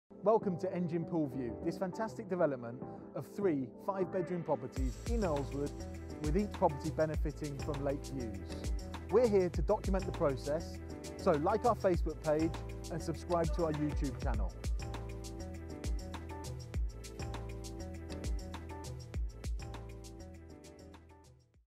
Earlswood - Engine Pool View - Earlswood Lakes - Solihull Property
An exciting new development that is launching in Earlswood! Three executive detached, five bedroom family homes with LAKE VIEWS!

For further enquiries, please contact Kathy or Lisa on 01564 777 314.